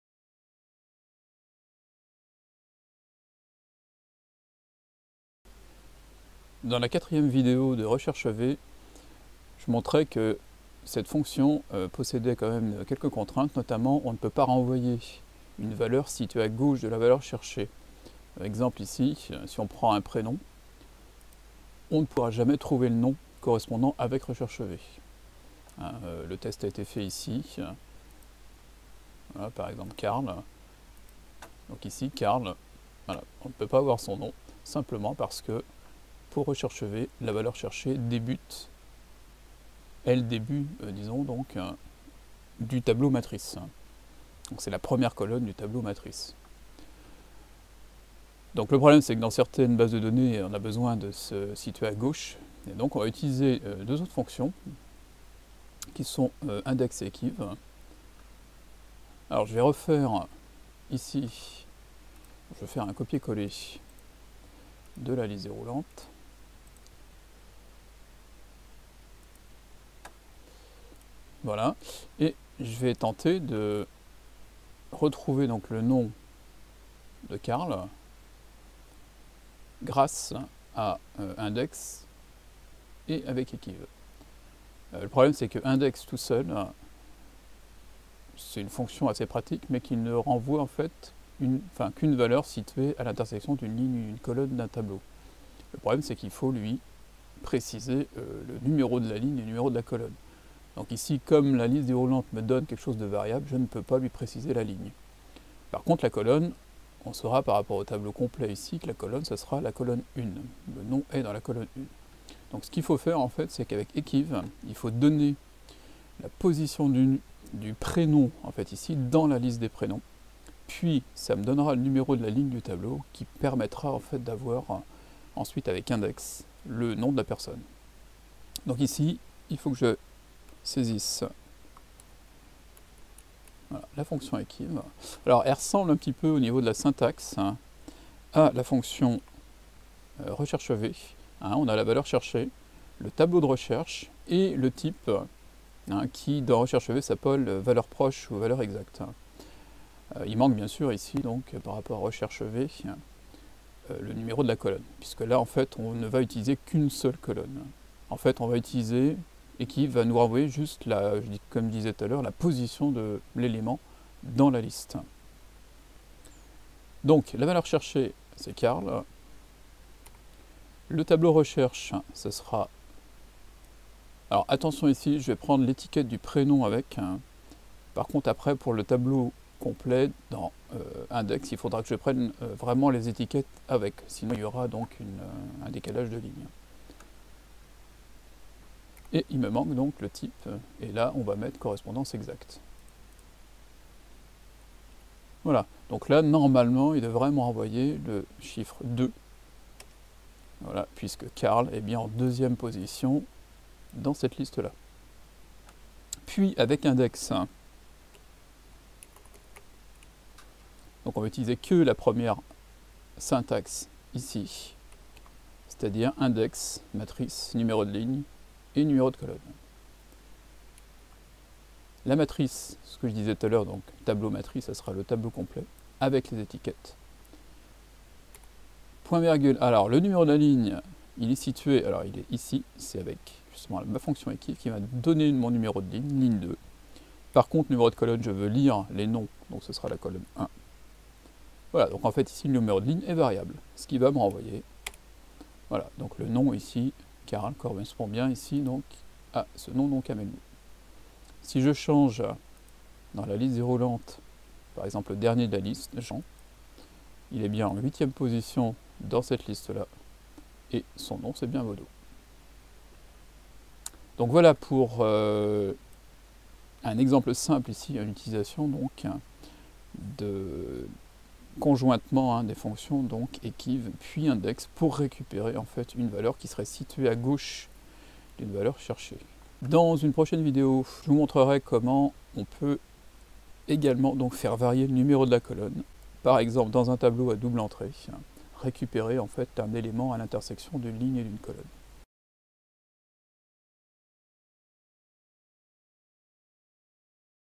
Excel 2010 - INDEX & EQUIV 1
Savoir utiliser EQUIV puis INDEX pour remplacer la fonction RECHERCHEV dans certains cas.
Exemple avec un numéro de ligne variable.